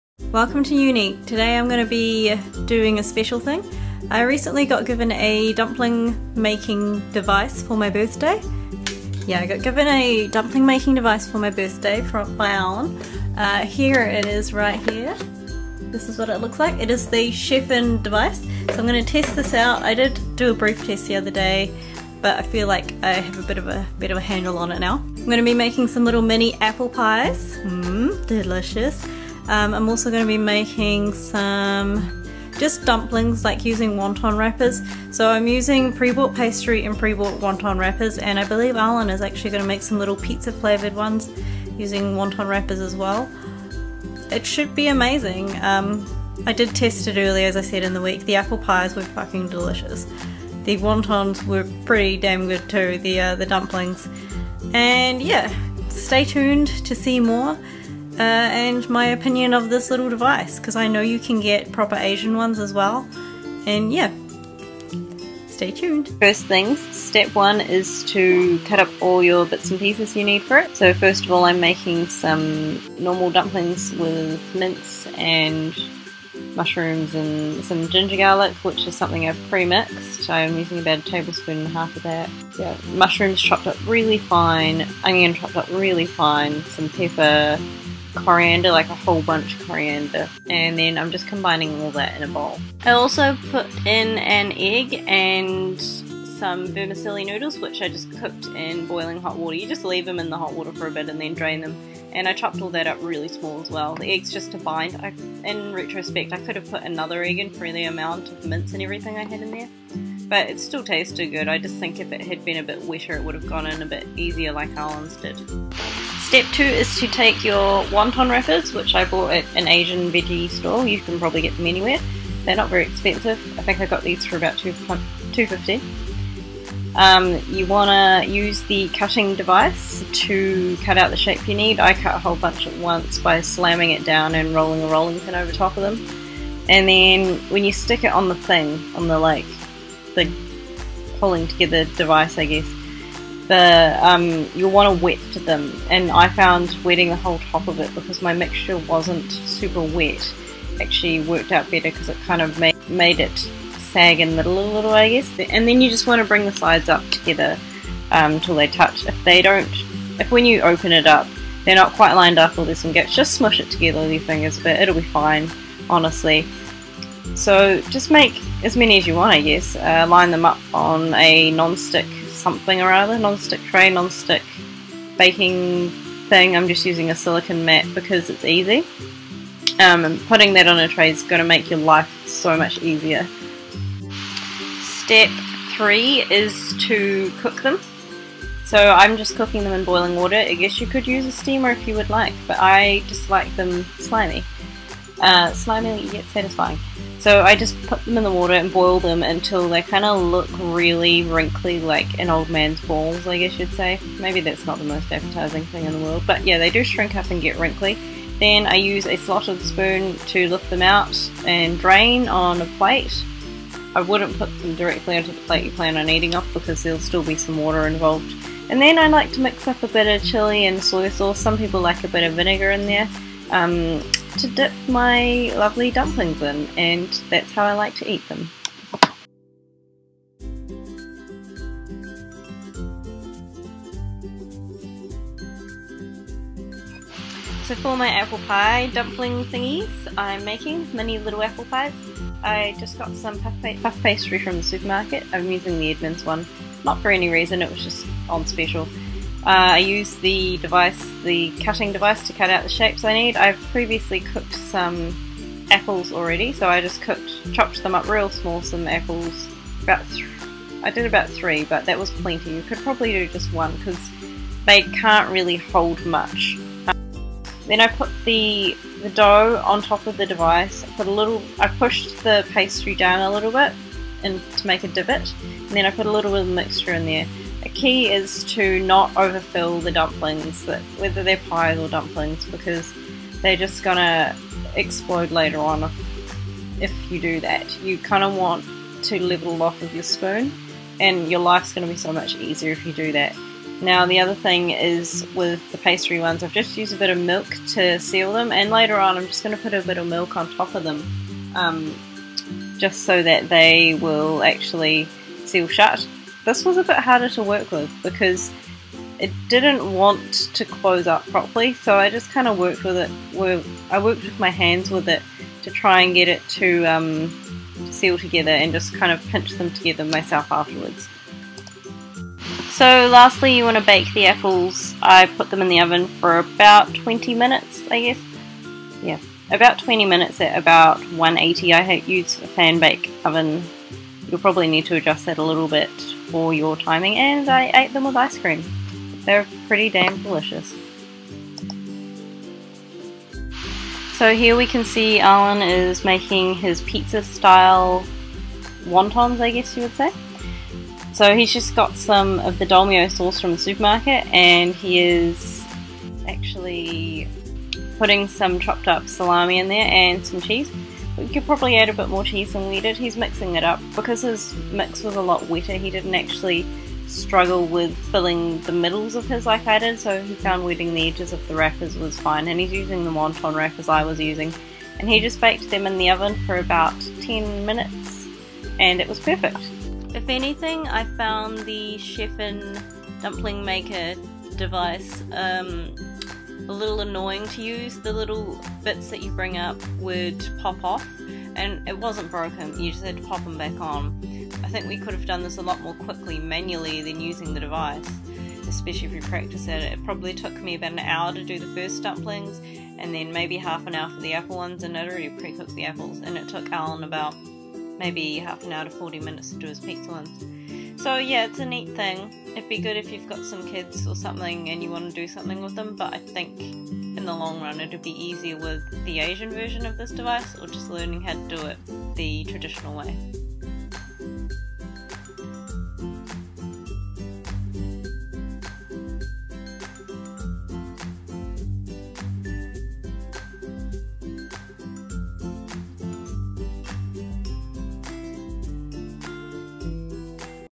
How to make dumplings using a chef'n pocket machine
Watch me make dumplings and also review the Chef'n pocket machine. See my recipe for dumplings and also some other uses for this kitchen gadget.